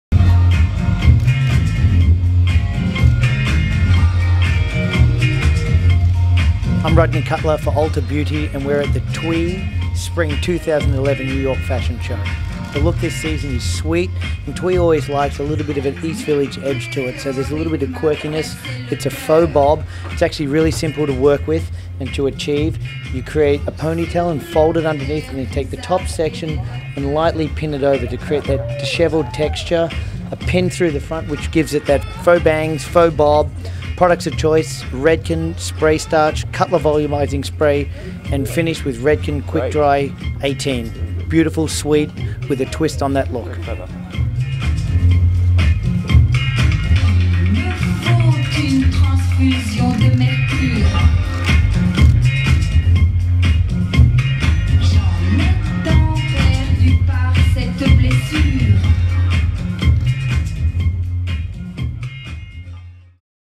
Cutler/Redken howto and Style, Thuy New York Fashion Week Spring 2011 interview.
Rodney Cutler talks hair backstage at the Thuy fashion show for Spring 2011
Filmed & Edited by Dean Holcombe